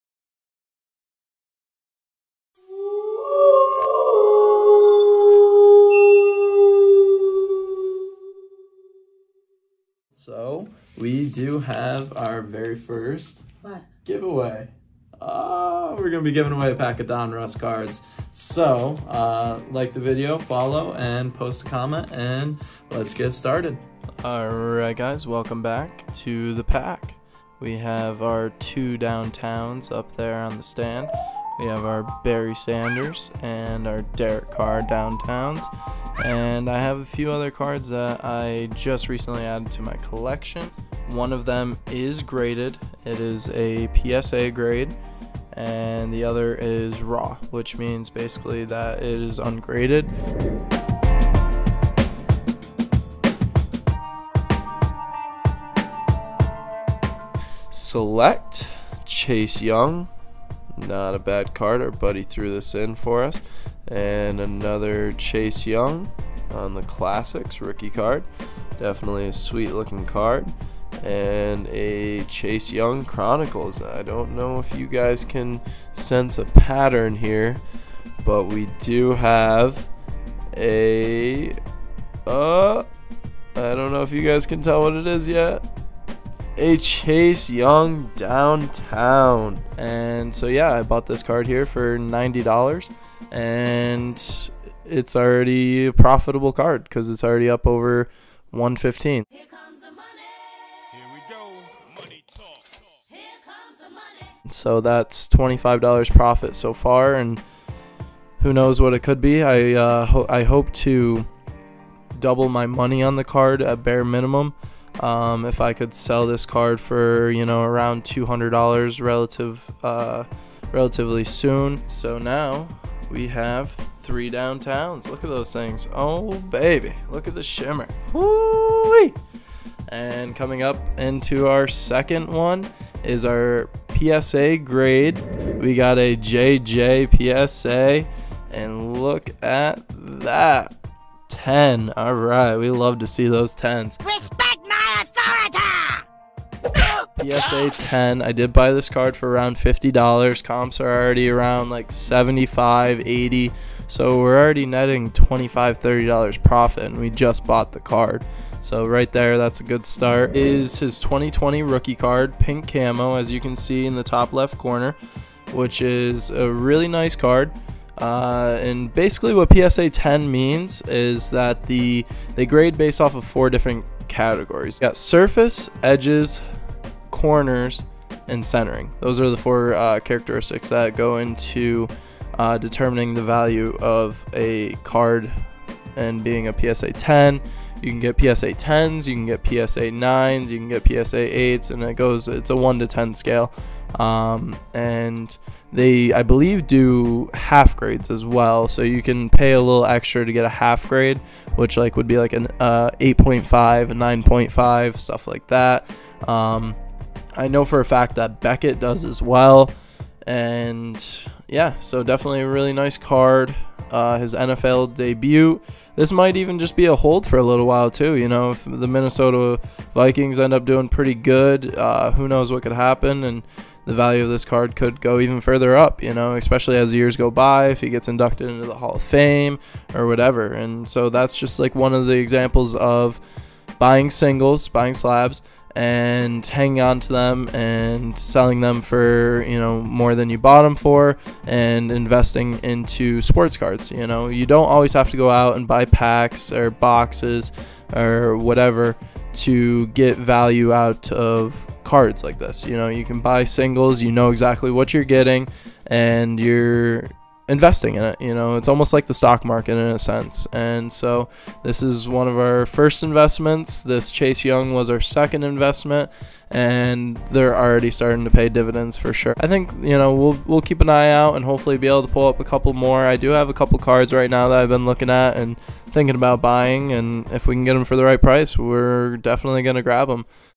📈Sports card investing 📈💰Doubling our money💰
Buying packs and breaking boxes isn’t the only way to go from rags to riches. You can invest into singles and slaps too. I’m already profitable on the cards!